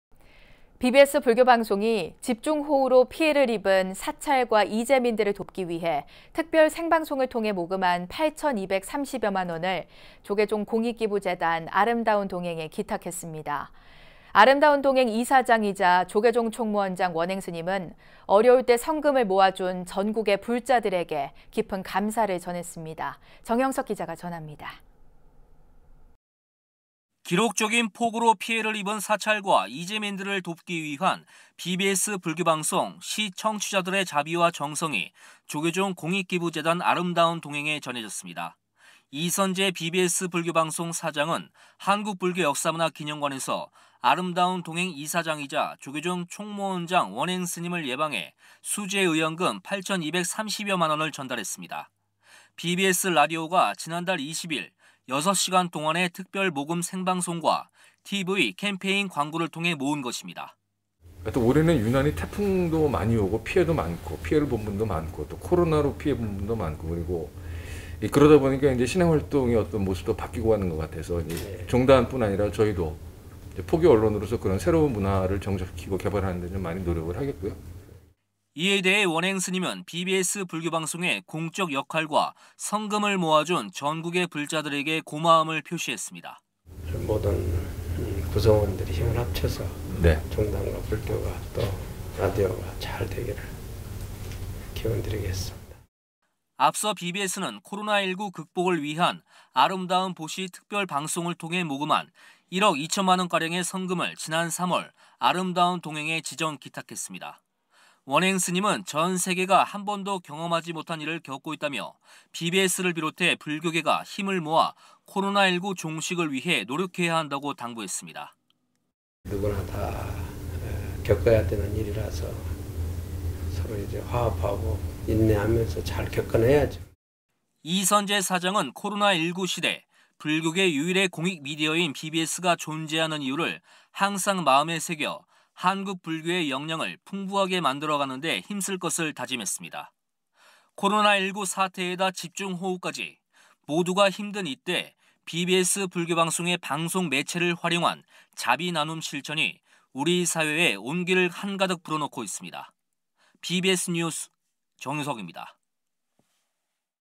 [BBS뉴스] 'BBS 수재민돕기 성금' 아름다운 동행에 전달
BBS 불교방송이 집중호우로 피해를 입은 사찰과 이재민들을 돕기 위해 특별 생방송을 통해 모금한 8,230여만 원을 조계종 공익기부재단 아름다운 동행에 기탁했습니다.

아름다운 동행 이사장이자 조계종 총무원장 원행스님은 어려울 때 성금을 모아준 전국의 불자들에게 깊은 감사를 전했습니다.

정영석 기자가 전합니다